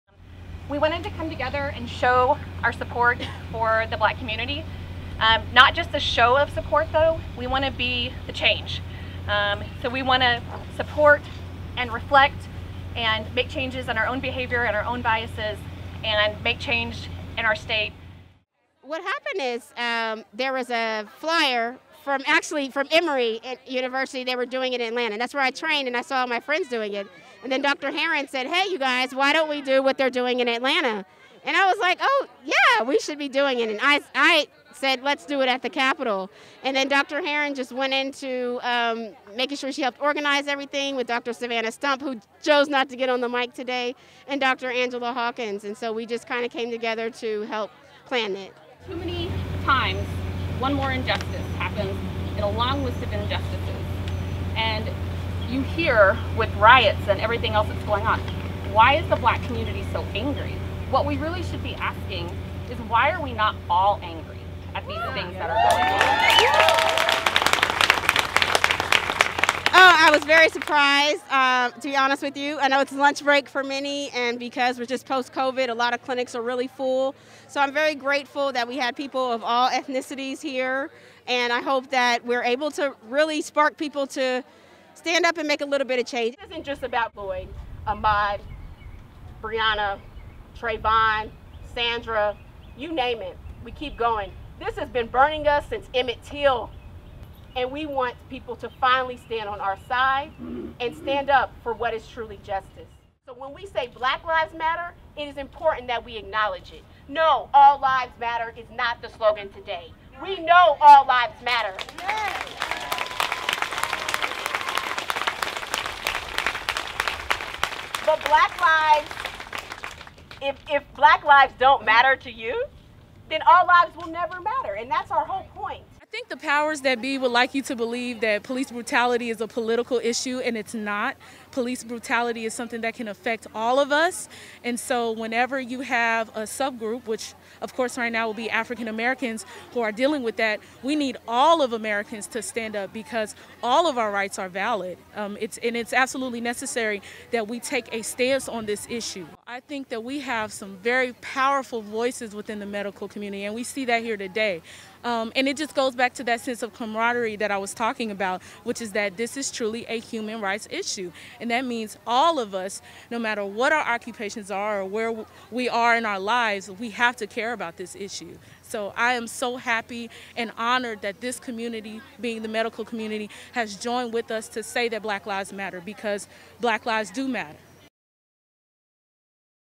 White coats for black lives
Hundreds of local medical professionals rallied at the Oklahoma state Capitol on Friday in a show of solidarity for those protesting police violence and racial injustice.